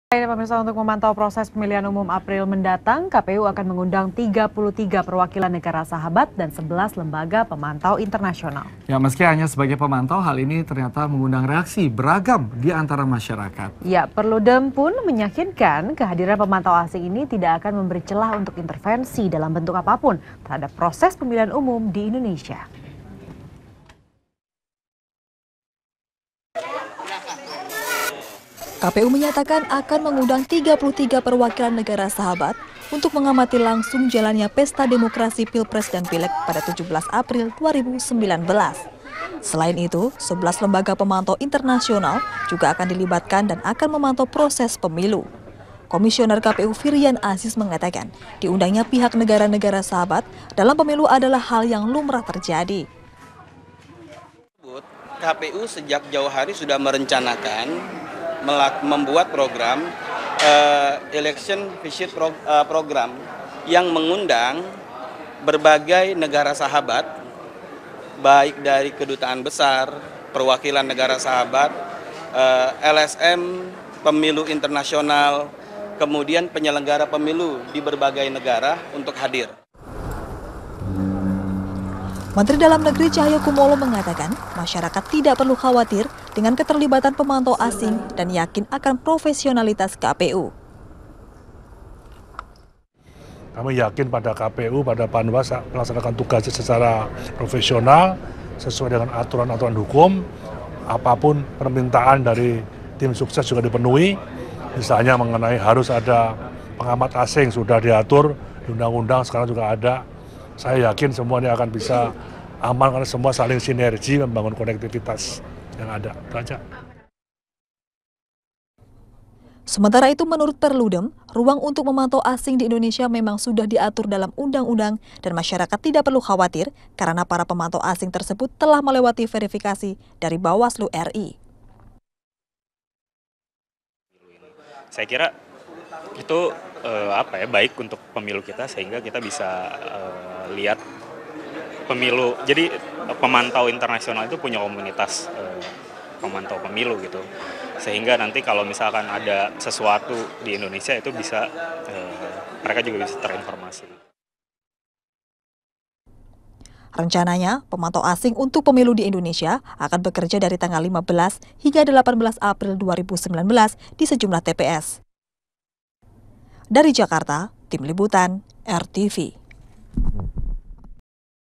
Pilpres 2019 Akan Diawasi Pemantau Asing
Pada Pemilu 2019  KPU akan mengundang 33 perwakilan negara sahabat dan 11 lembaga pemantau internasional  untuk mengamati langsung jalannya pemilu 2019.